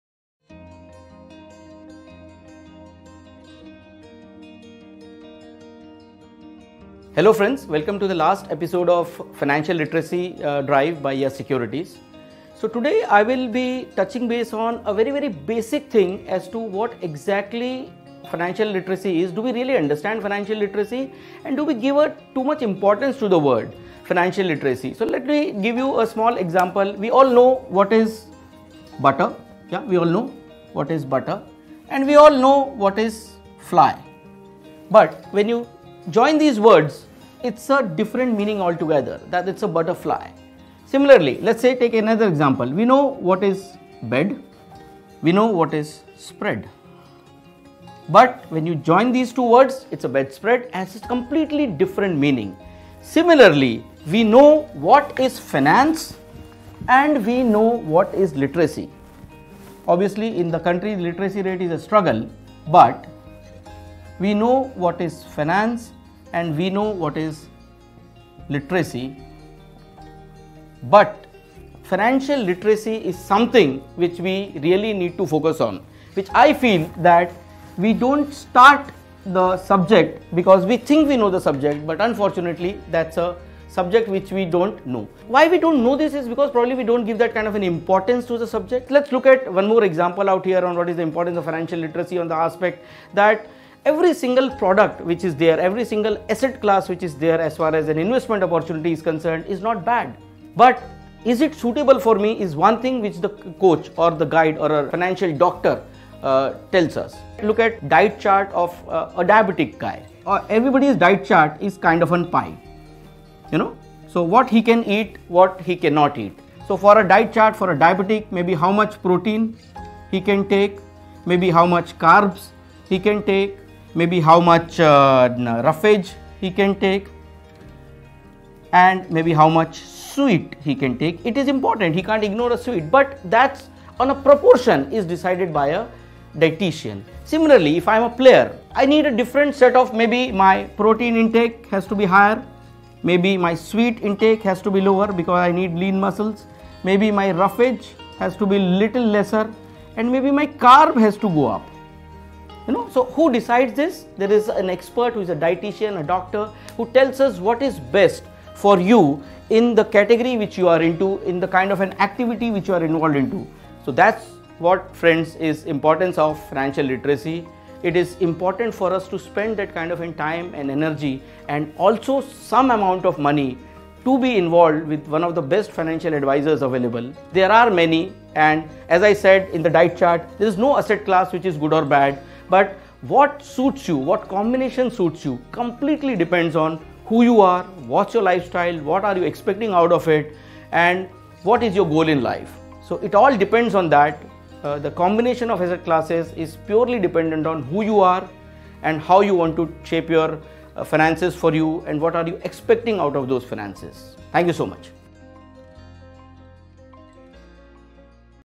FinancialLiteracyWeek|| Importance of financial literacy
Watch Mr. Anshul Arzare, Chief Business Officer, YES SECURITIES, share his views on what is financial literacy and why it is important for everyone to be financially literate.
He touches upon how we aspire to hire the best professionals to handle aspects of our lives such as education, fitness etc., but we fail to apply the same when it comes to handling our finances; especially with regards to the importance of a financial planner.